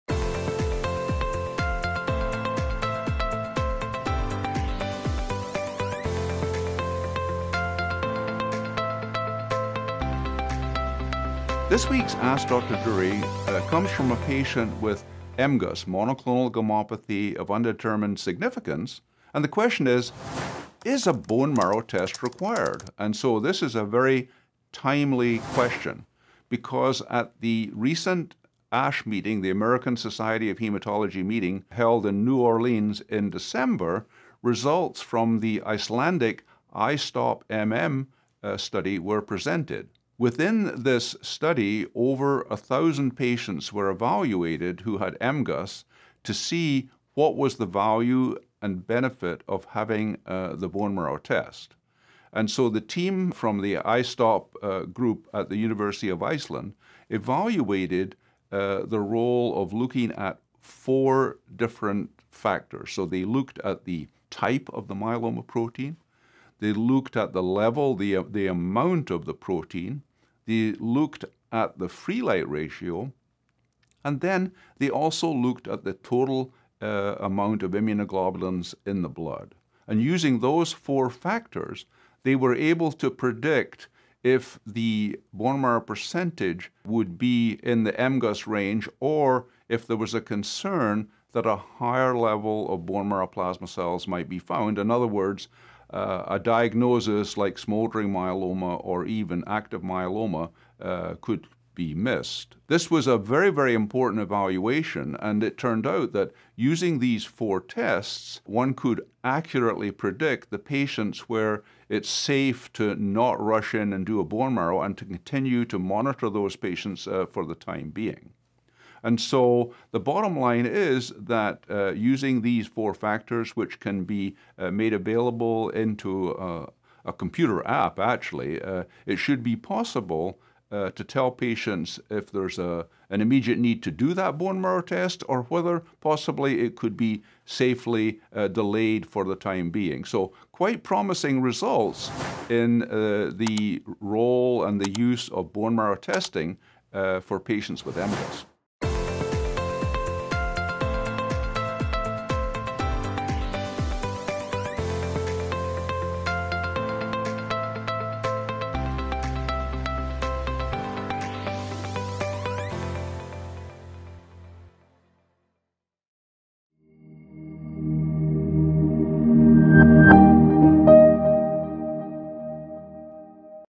Do patients with MGUS need to have a bone marrow test?
In this week’s AskDrDurie, myeloma expert Dr. Brian G.M. Durie discusses when MGUS patients should undergo bone marrow biopsy and how the IMF’s iStopMM study results—presented at the 2022 American Society of Hematology (ASH) meeting—may help predict when a bone marrow biopsy is needed.

The BOTTOM LINE: Using the iStopMM model, bone marrow sampling may be safely deferred. However, other studies will be needed for validation.

Category
Nonprofits & Activism
License